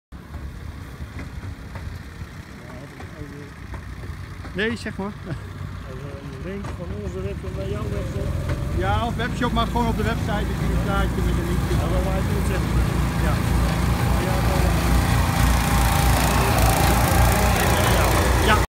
Paddock Cleaner mestveger achter minitrekker van manege Leiden
Paddock Cleaner mestveger achter minitrekker van manege Moedig Voorwaarts in Leiden.